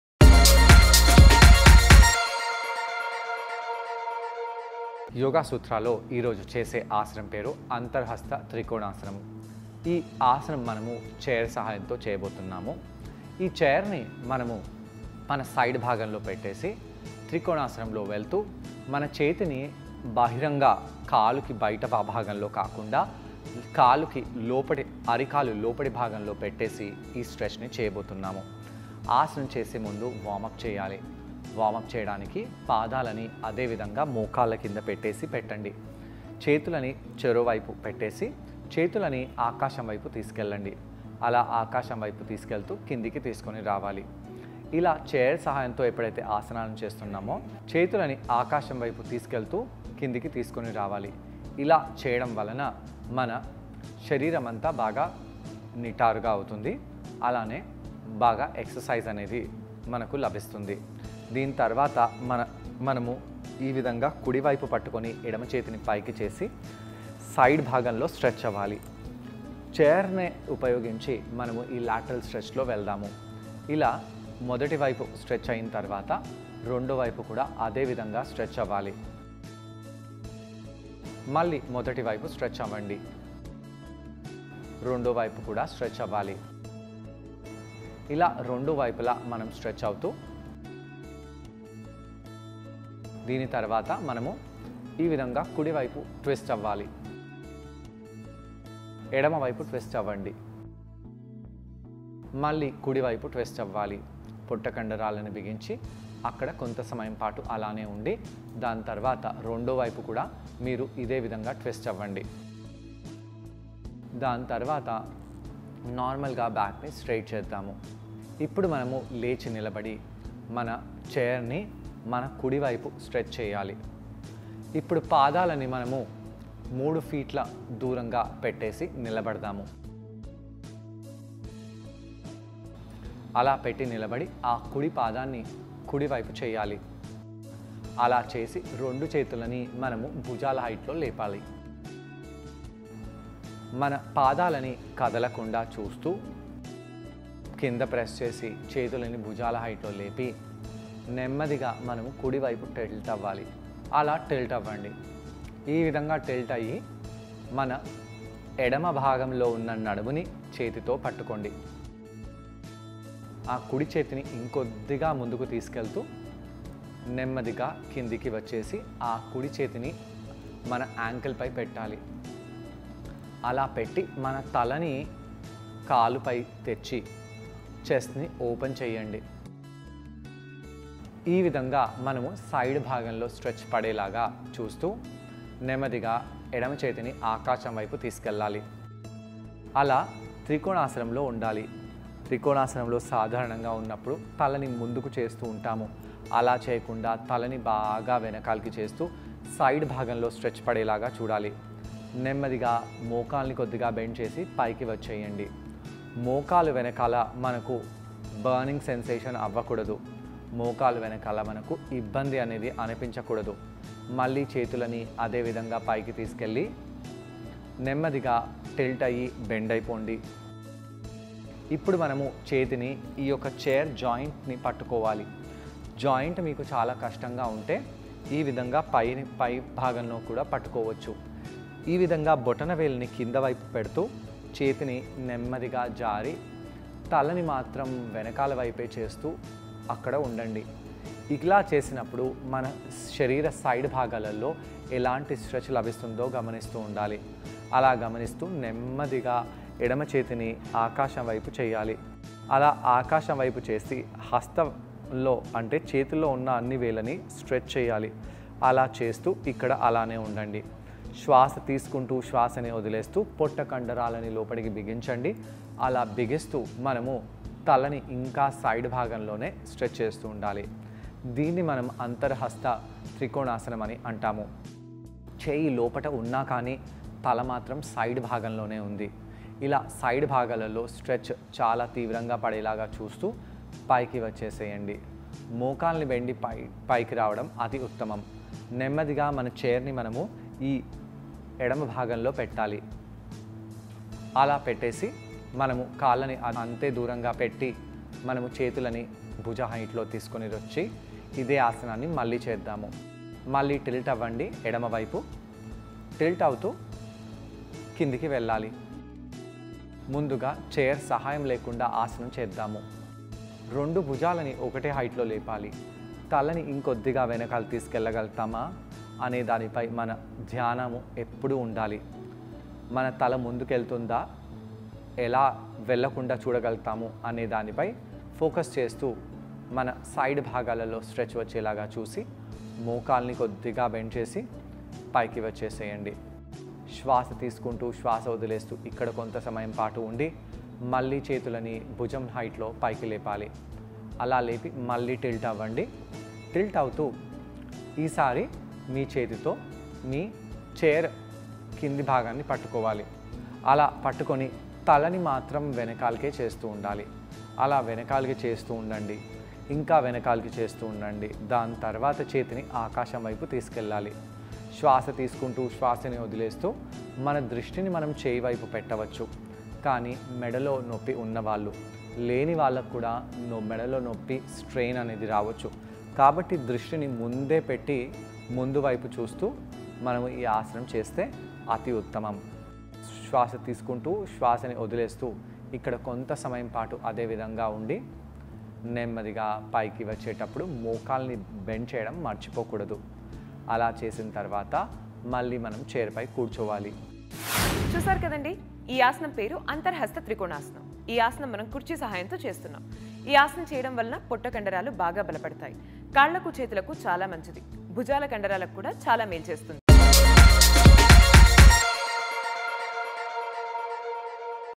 Anthara Hasta Trikona Asana (With Chair) | అంతర హస్త త్రికోణాసనం | Yoga Sutra | 19th May 2021
అంతర హస్త త్రికోణాసనం

The instructor demonstrates the necessary steps to do Anthara Hasta Trikona Asana by using a chair. This asana helps to increase blood circulation in the head.